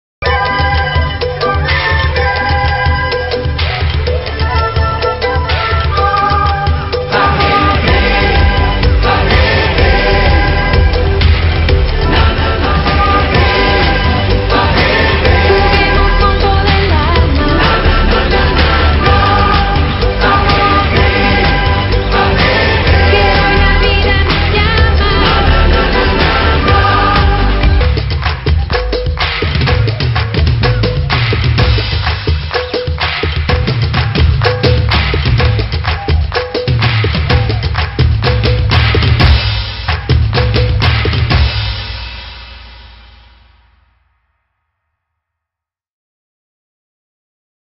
Hockey sobre césped. Entrenamiento camino a Lima
Práctica del deporte en la Escuela Nacional de Hockey sobre césped Antonio Maceo